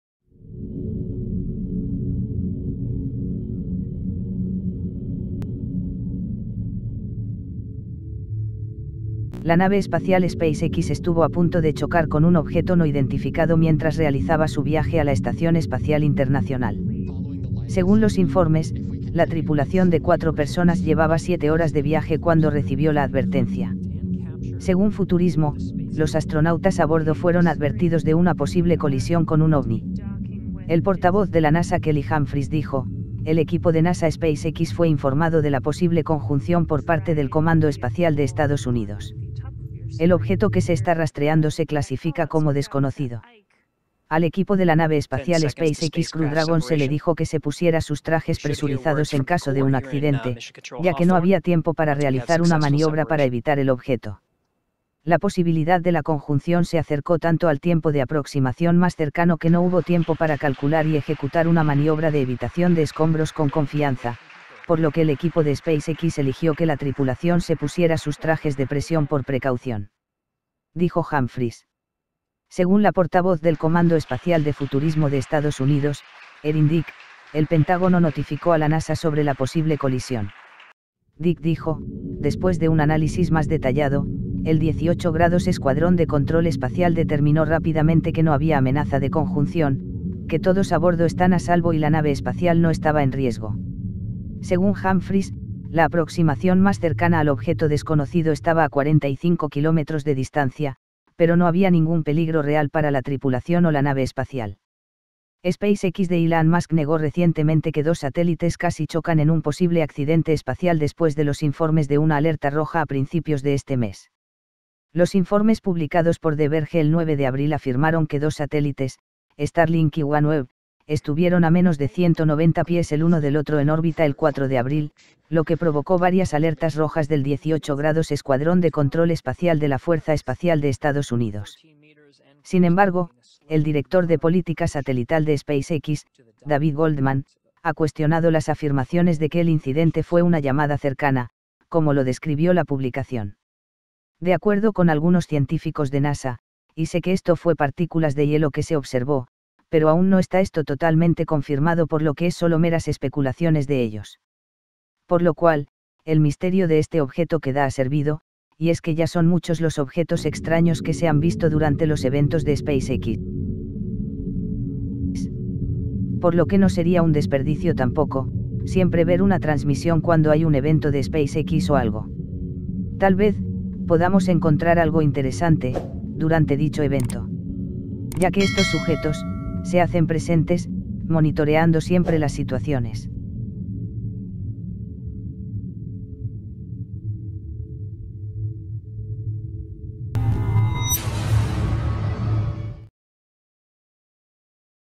Un cohete de SpaceX casi se impacta con un OVNI, revela la NASA (Video)
Un objeto espacial no identificado estuvo a punto de colisionar con la nave de la empresa del multimillonario Elon Musk con cuatro astronautas a bordo. Imágenes del lanzamiento muestran el momento en el que pasa a su lado.